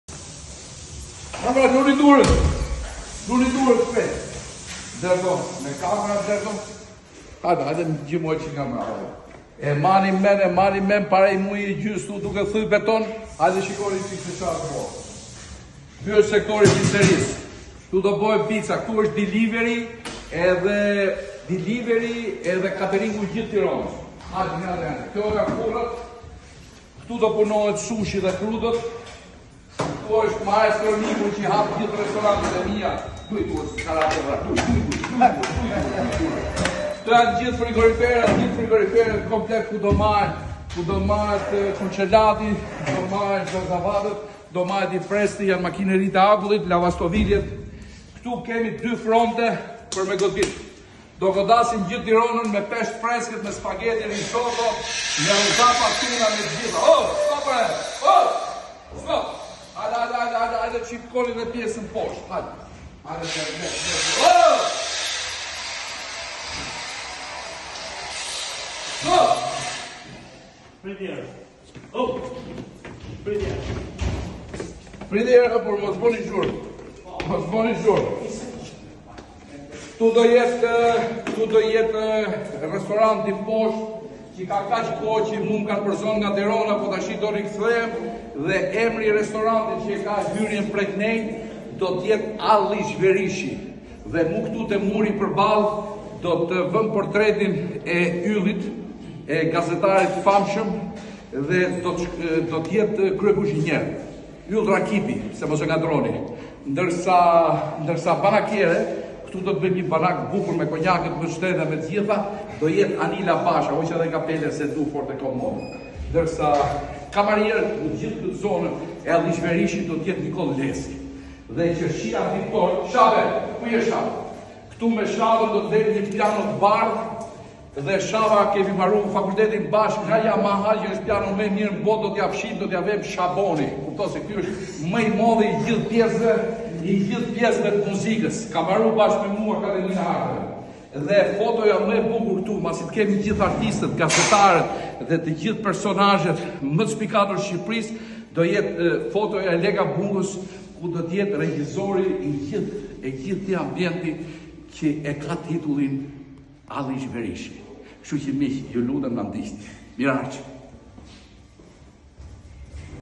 Gjergj Luca: Rozafa mendon për çdo individ e për çdo familje në Tiranë
Tani e tutje peshku dhe prodhimet e detit nuk do jenë më ushqim ekskluziv vetëm për njerëzit e pasur. Rozafa mendon për çdo individ e për çdo familje në Tiranë, Rozafa detin do ua sjellë në shtëpitë tuaja...
Rozafa Delivery, hapni dyert se po vimë!